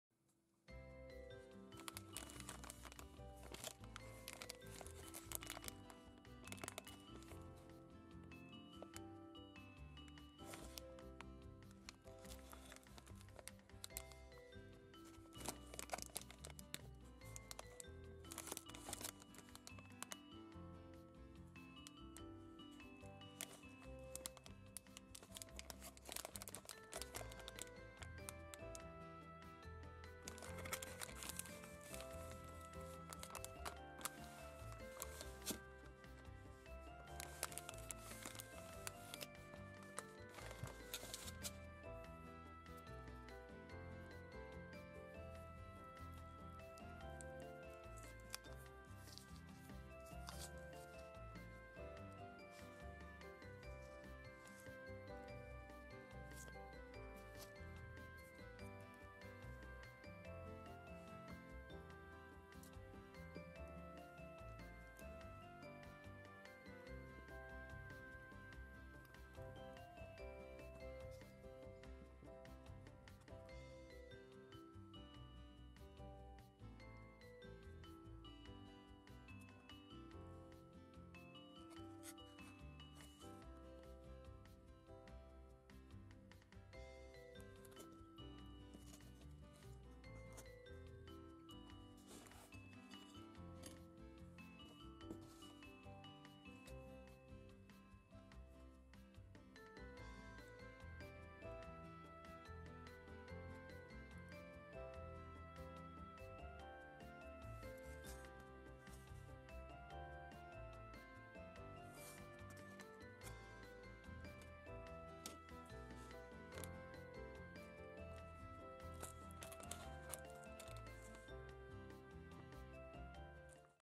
鬼滅の刃 魚肉ソーセージ Kimetsu no Yaiba Sausage(Demon Slayer)
An original card from "Kimetsu no Yaiba" is included as an extra.

Contains plenty of calcium, a food with nutrient function.
It is perfect for children who are growing up.